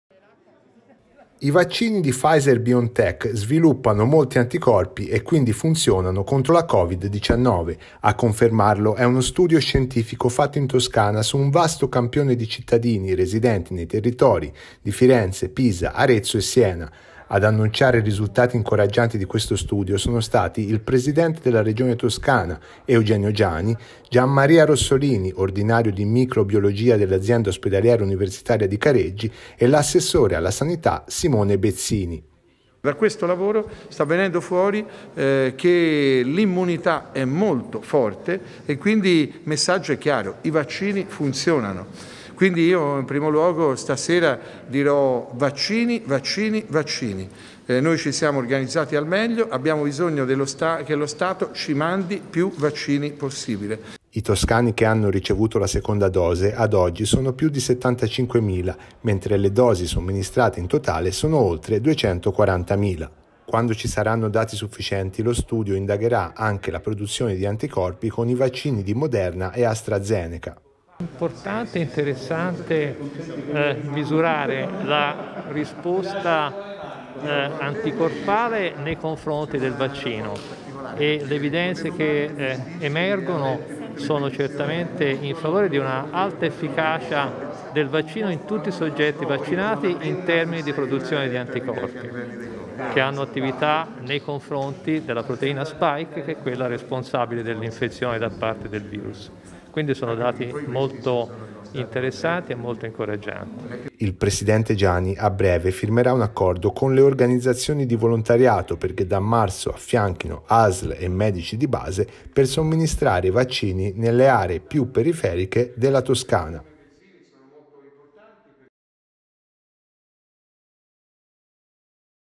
L' efficacia dei vaccini Pfizer-BioNTech
Elevata risposta immunitaria  e alta produzione di anticorpi nei soggetti vaccinati. Lo studio condotto in Toscana evidenzia l'efficacia dei vaccini Pfizer-BioNTech rilevata in un vasto campione di cittadini residenti nelle province di Firenze, Arezzo, Pisa e Siena. I toscani che hanno ricevuto la seconda dose di vaccino sono ad oggi più di settantacinquemila, mentre le dosi somministrate sono in totale oltre duecentoquarantamila.Quando il numero delle somministrazioni raggiungerà un livello adeguato ad avviare una indagine su un campione significativo di cittadini vaccinati con altri tipi di siero sarà avviato un ciclo di test anche sui preparati forniti da AstraZeneca e Moderna per misurarne l'efficacia.